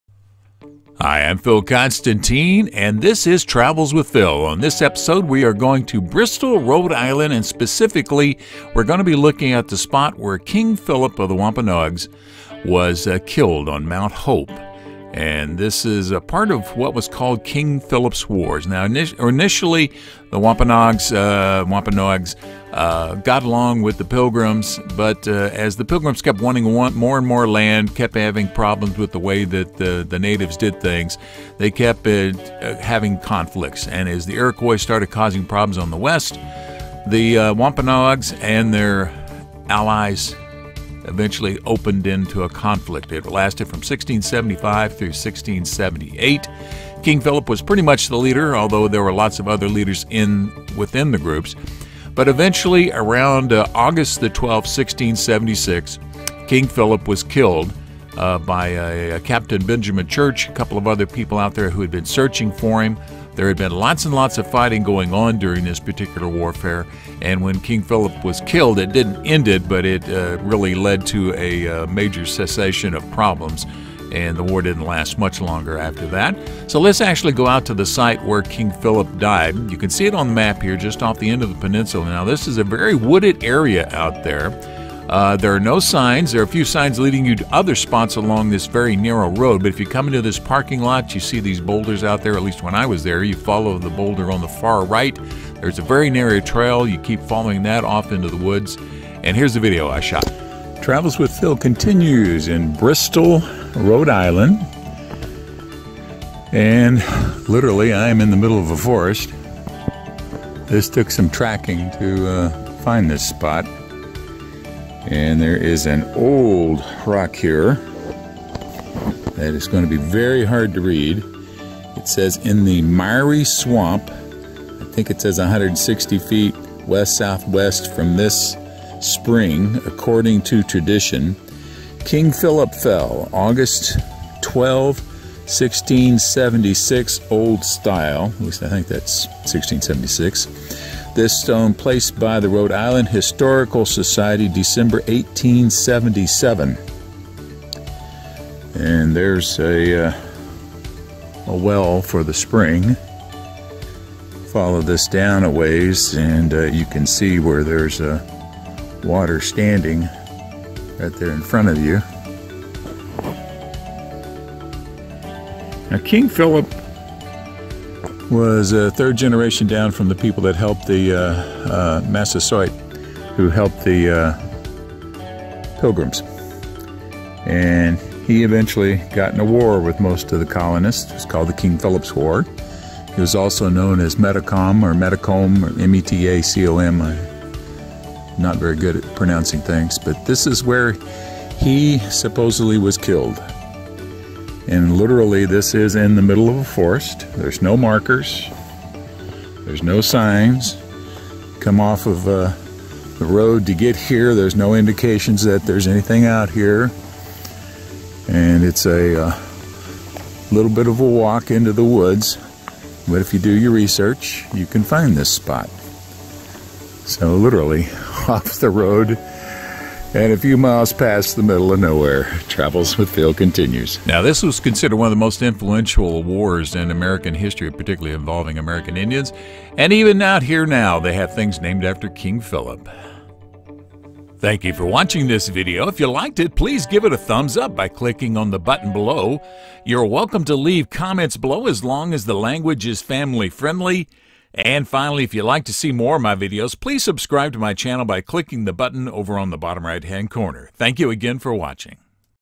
King Philip's Death Site - Bristol, Rhode Island - Travels With Phil
King Philip's Death Site - Bristol, Rhode Island - Metacomet was also known as King Philip. He was a leader of the Wampanoag tribe. After some peaceful co-existance with the Pilgrims, conflict broke out in 1675. It became known as King Philip's War.  This video goes to where King Philip was killed. Travels With Phil
From Wikipedia:
Metacom (1638–1676), also known as Metacomet and by his adopted English name King Philip, was chief to the Wampanoag people and the second son of the sachem Massasoit.

King Philip's War (sometimes called the First Indian War, Metacom's War, Metacomet's War, Pometacomet's Rebellion, or Metacom's Rebellion) was an armed conflict in 1675–78 between Indian inhabitants of New England and New England colonists and their Indian allies. The war is named for Metacomet, the Wampanoag chief who adopted the name Philip because of the friendly relations between his father Massasoit and the Mayflower Pilgrims. The war continued in the most northern reaches of New England until the signing of the Treaty of Casco Bay in April 1678.

Metacomet's allies began to desert him, and more than 400 had surrendered to the colonists by early July. Metacomet took refuge in the Assowamset Swamp below Providence, and the colonists formed raiding parties of militia and Indian allies. Metacomet was killed by one of these teams when he was tracked down by Captain Benjamin Church and Captain Josiah Standish of the Plymouth Colony militia at Mount Hope in Bristol, Rhode Island. He was shot and killed by an Indian named John Alderman on August 12, 1676. Metacomet's corpse was beheaded, then drawn and quartered, a traditional treatment of criminals in this time period. His head was displayed in Plymouth for a generation.

Captain Church and his soldiers captured Pocasset war chief Anawan on August 28, 1676 at Anawan Rock in Rehoboth, Massachusetts. He was an old man at the time, and a chief captain of Metacomet. His capture marked the final event in King Philip's War, as he was also beheaded.
Travels with Phil copyrighted by Phil Konstantin -
Photo Credit (other than my own):
The history of King Philip's war _ Church, Benjamin, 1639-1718-PD = =
In King Philip's war_ a story of two boys captured by the great sachem _ Frederick Ober-1907-PD = =
Pictorial history of King Philip's war; _ Strock, Daniel, Jr _ 1853-PD = =
The Project Gutenberg eBook of King Philip, by John S. C. (John Stevens Cabot)-PD = =
death-of-king-philip-18571-PD = =
Engraving depicting the colonial assault on the Narragansetts' fort in the Great Swamp Fight in December 1675-Wikipedia-PD = =
Indians Attacking a Garrison House, from an Old Wood Engraving This is likely a depiction of the attack on the Haynes Garrison, Sudbury, April 21, 1676-Wikipedia-PD = =
King Philip Dying for His Country-PD = =
Map2-King Philip's Death Site-Bristol RI-41°40'19.1_N 71°14'39.9_W - Google Maps = =
Philip. King of Mount Hope, a 1772 engraving of a caricature of King Philip by Paul Revere-Wikipedia-PD = =
Pictorial history of King Philip's war-Daniel Strock Jr-1853-PD = =
Tribal_Territories_Southern_New_England-PD -